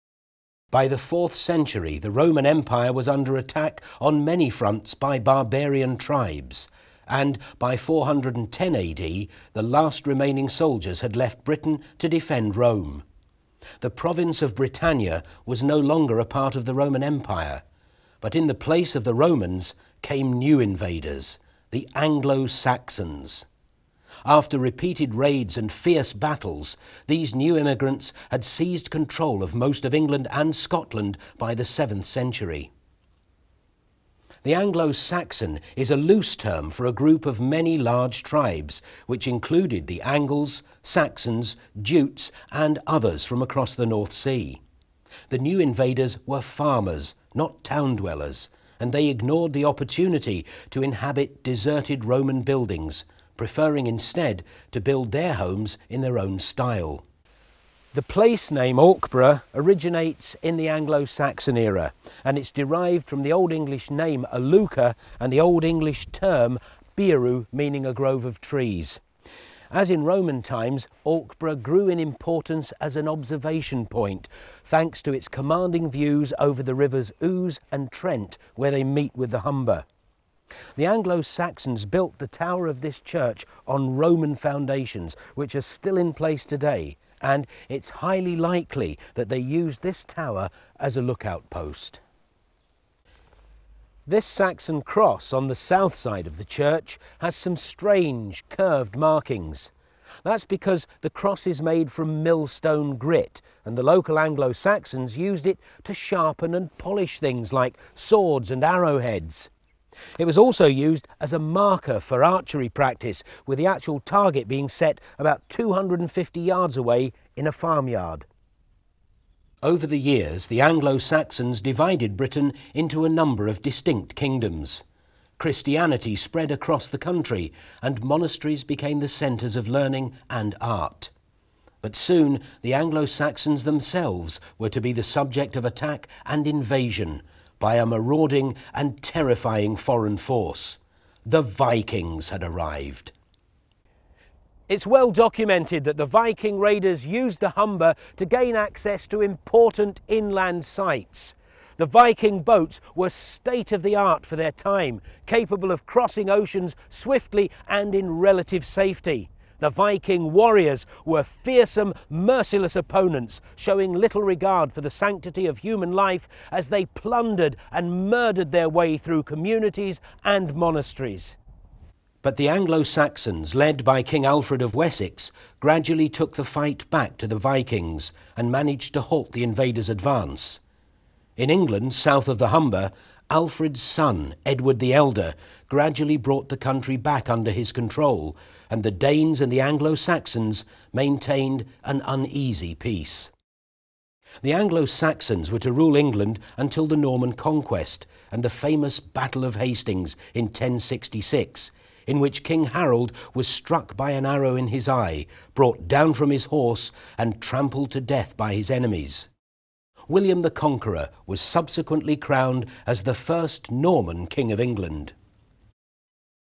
Alkborough Flats Project - CD-ROM Clip 7 of 13
Alkborough is a village of about 455 people in North Lincolnshire, England, located near the northern end of The Cliff range of hills overlooking the Humber Estuary at the Trent Falls, the confluence of the River Trent and the River Ouse.

This video clip is taken from a CD-ROM that was produced about the village itself and the proposed Alkborough Flats Project, a flood defense initiative.

More information about the Alkborough Flats Project can be found by searching Online or by visiting one of the following links, (which were correct and working at the time of publishing this video):